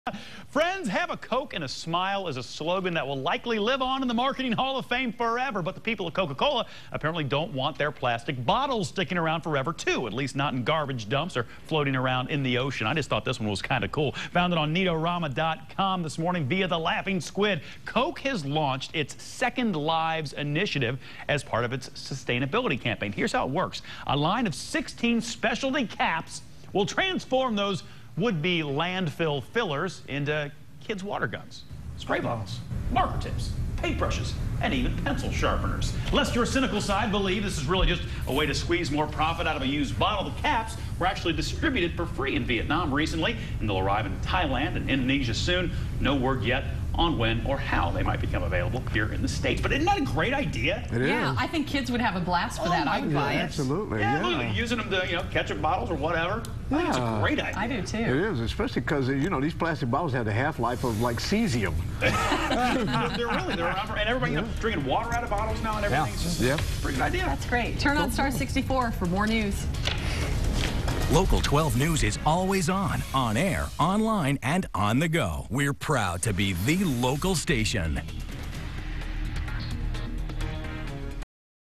The Cooler: Cool Coke caps
Coca-Cola repurposes plastic bottles